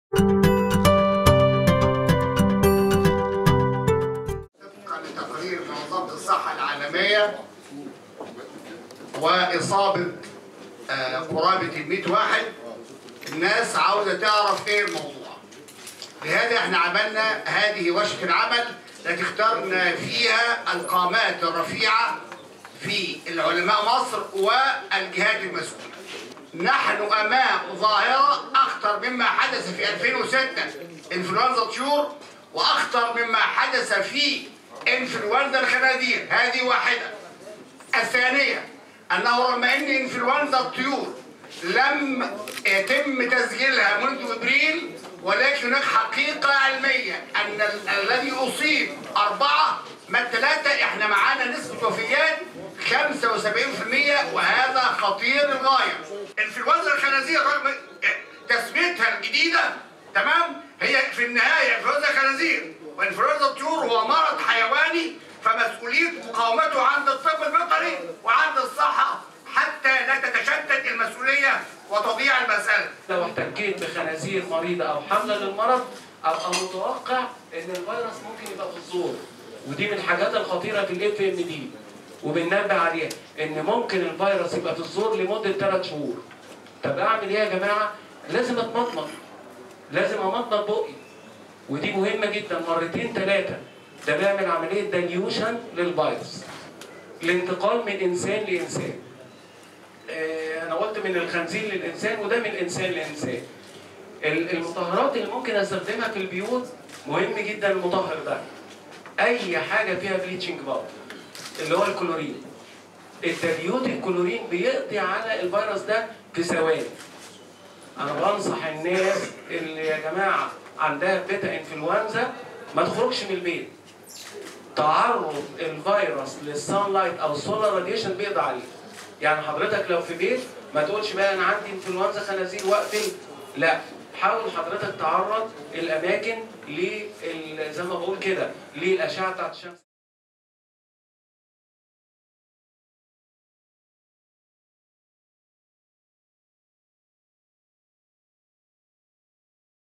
نقيب الأطباء: نحن أمام ظاهرة أخطر مما حدث في ٢٠٠٦ من إنفلونزا الطيور والخنازير
عقدت اللجنة العلمية للأطباء البيطريين اليوم ٤ فبراير ٢٠١٤ ورشة عمل حول انفلونزا الطيور والخنازير، أعلن خلالها نقيب الأطباء ان مصر أمام ظاهرة أخطر مما حدث في سنة ٢٠٠٦ من انفلونزا .